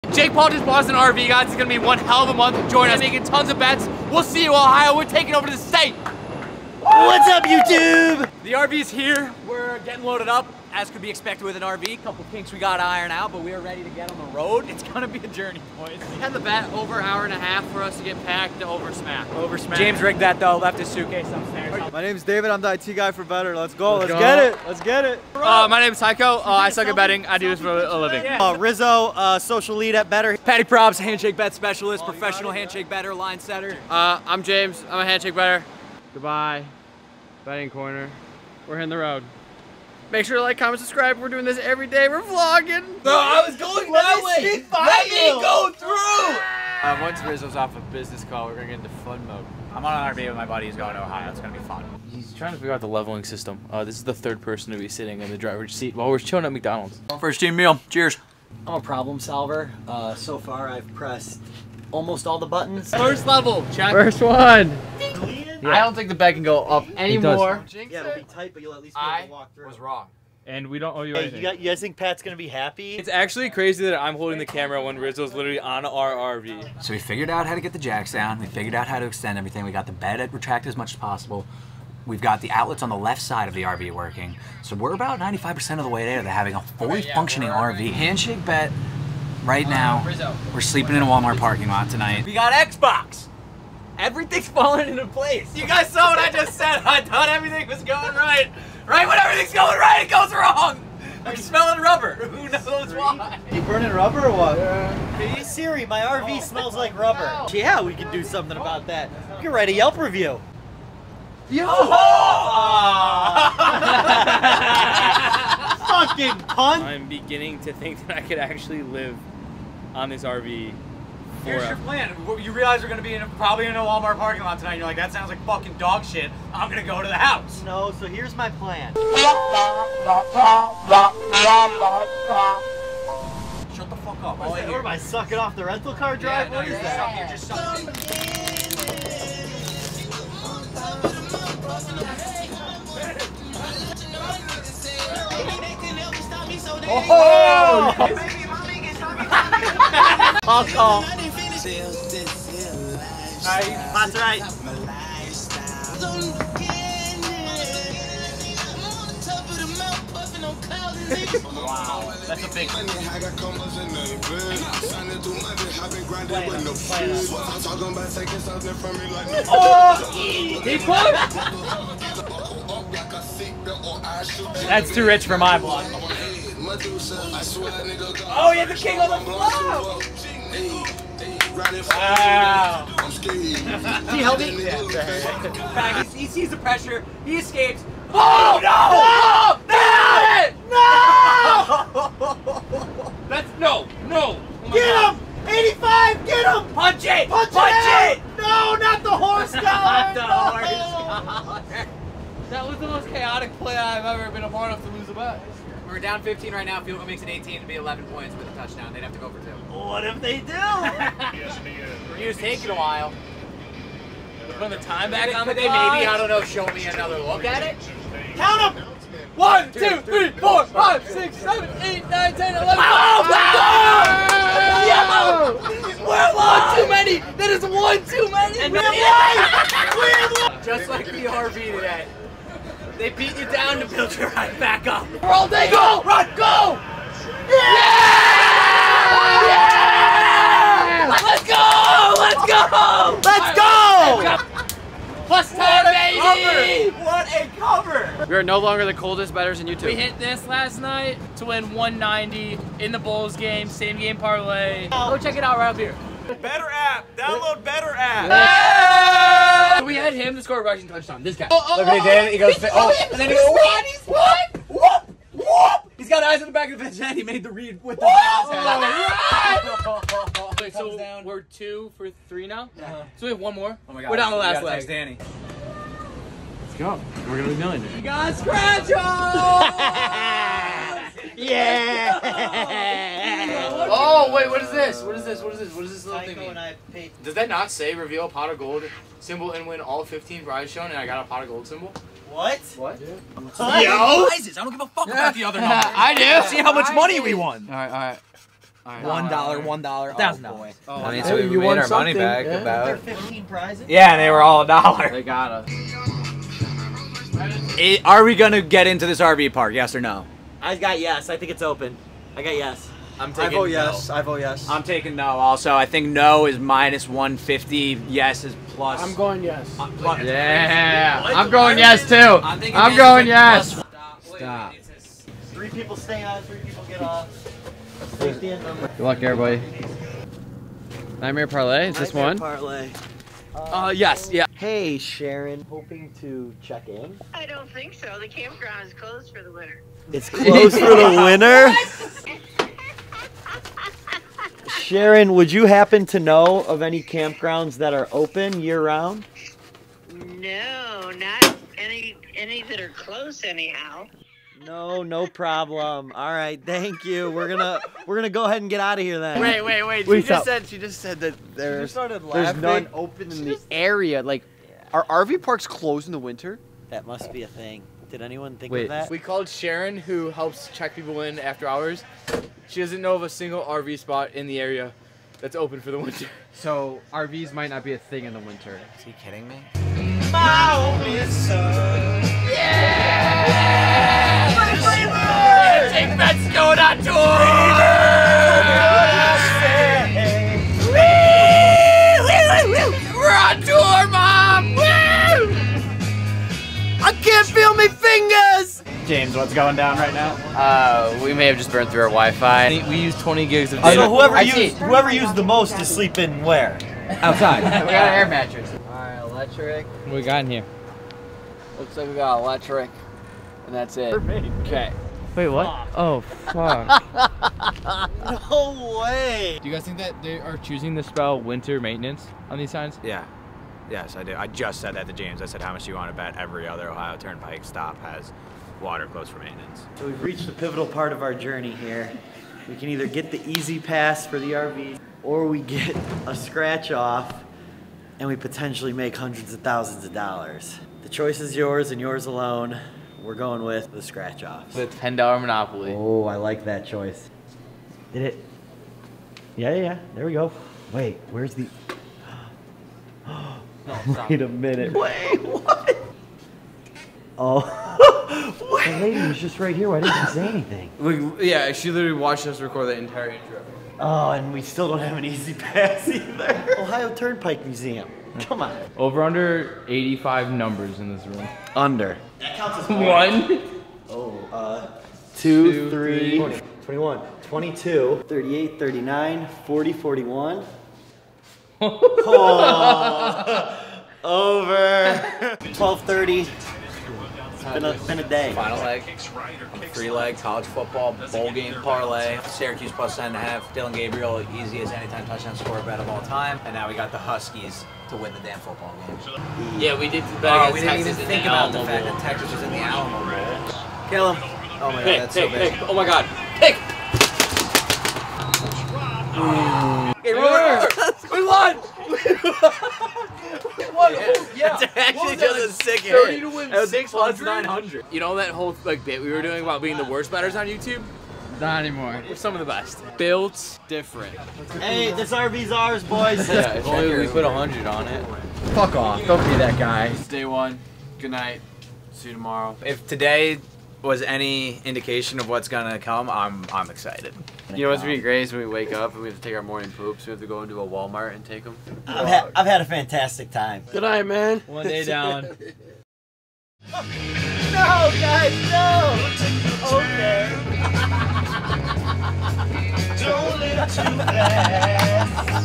JAKE PAUL GAVE HANDSHAKE BETS AN RV TO LIVE IN OHIO FOR THE MONTH TO LAUNCH THE BETR APP
Handshake Bets got gifted an RV from Jake to go on their mission to get Ohioans to fall in love with microbetting! Here is the first day recap of their journey - let's just say there were many highs and lows.

If you or someone you know has a gambling problem, call or text the National Problem Gambling Helpline Network at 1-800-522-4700 for confidential help.